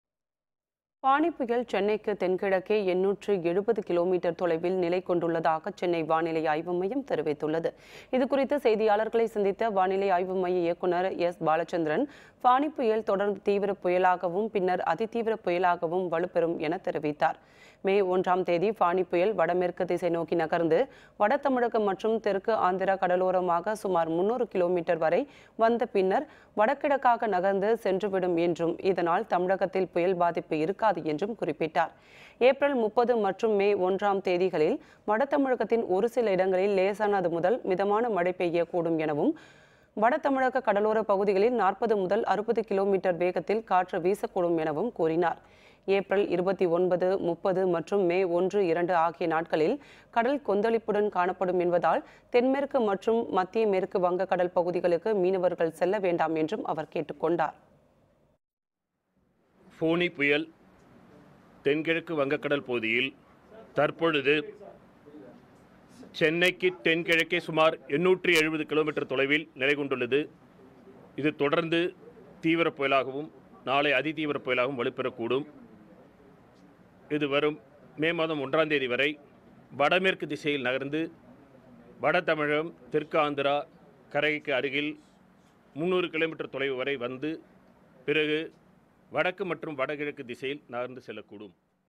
ஃபானி புயல் தீவிர புயலாக வலுப்பெறும் வானிலை ஆய்வு மையம் தகவல் -NewsBite(29/04/19)
ஃபானி புயல் தீவிர புயலாக வலுப்பெறும் வானிலை ஆய்வு மையம் தகவல்